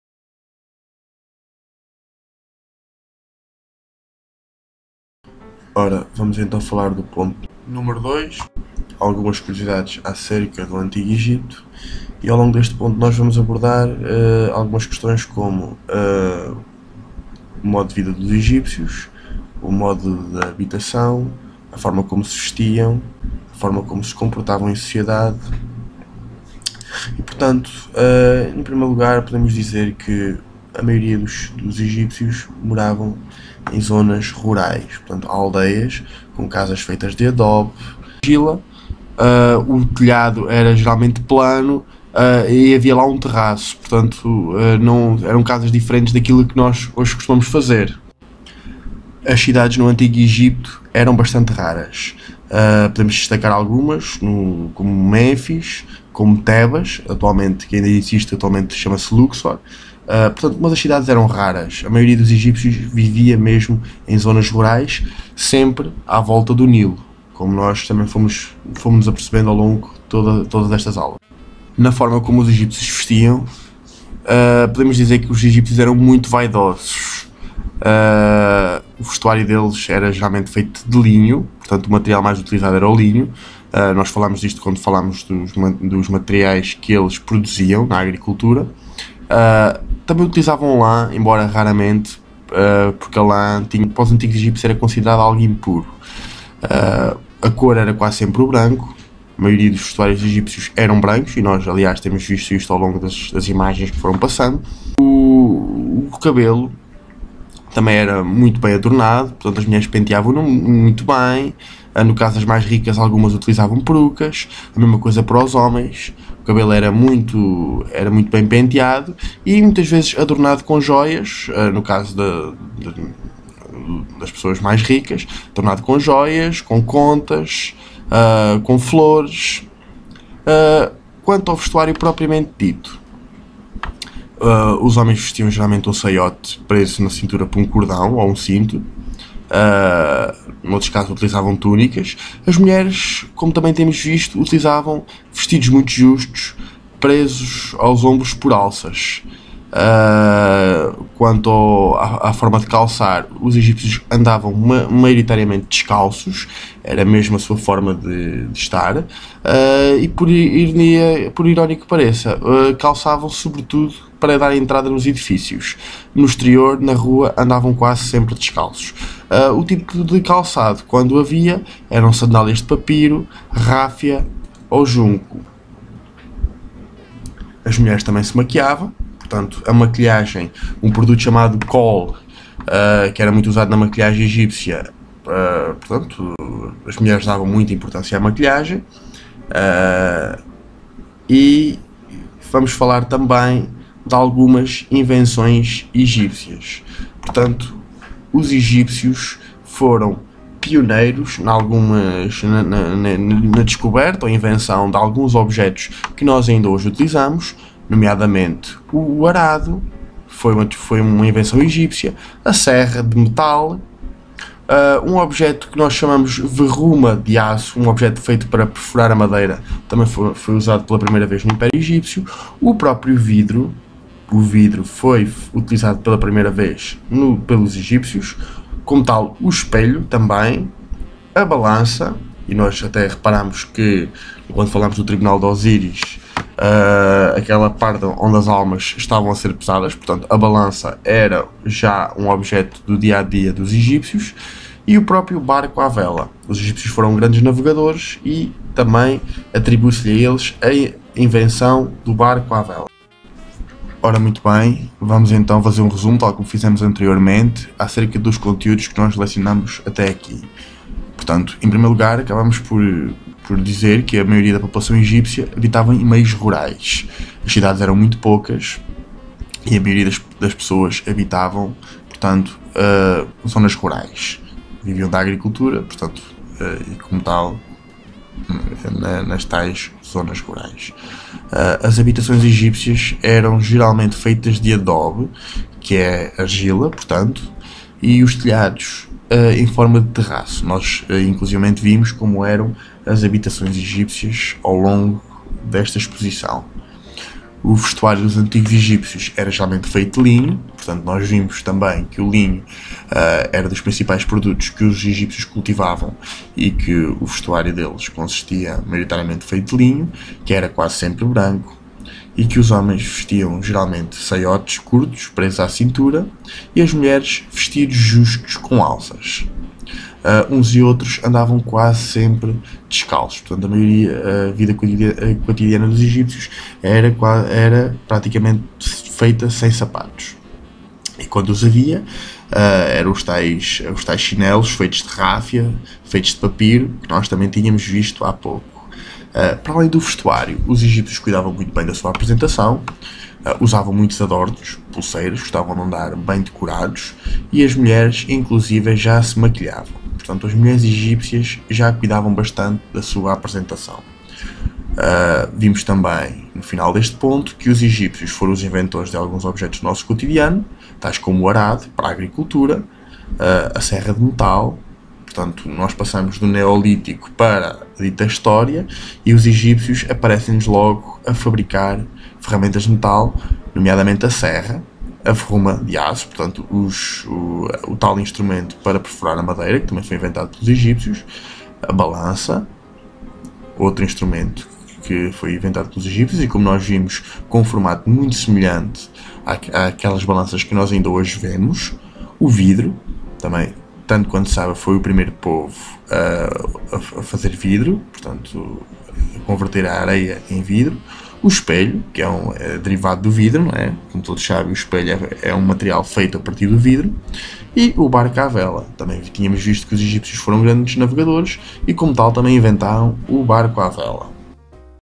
13 - Curiosidades Sobre o Antigo Egipto (excerto) (História, 7º Ano)
A fim de tornar as aulas mais MEGA apelativas e mais MEGA fáceis de consultar, serão criadas versões em excertos.

Ao longo deste excerto, os alunos poderão aprender umas breves curiosidades acerca da Civilização Egípcia, tais como o tipo de habitações, a forma de vestir e algumas invenções egípcias ainda usadas na nossa sociedade.